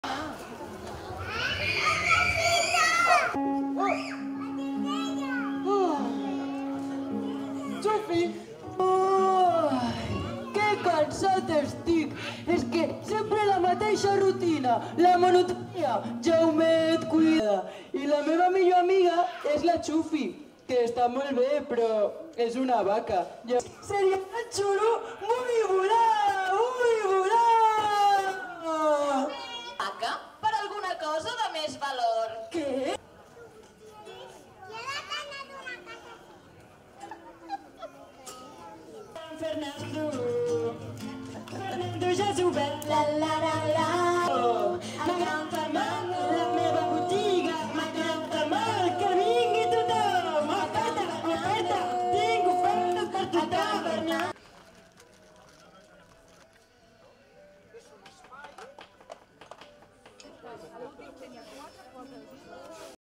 El Festival Didó tanca la vuitena edició apostant pels titelles amb música en directe
El Festival Didó ha tancat la vuitena edició després de tres dies d’activitats